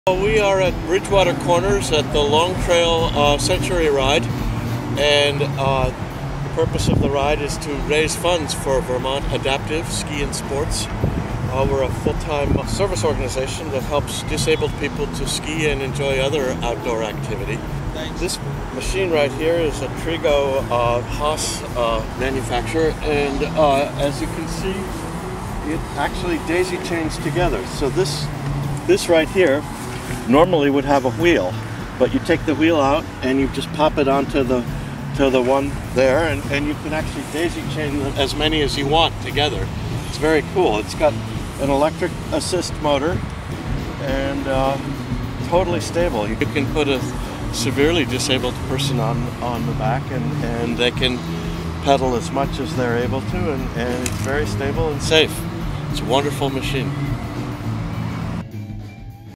Hase Trigo & Trigo Up Recumbent Trikes in Tandem
Check out this Hase Trigo & Trigo Up tandem combination spotted at the Long Trail Century Ride to benefit Vermont Adaptive Ski & Sports!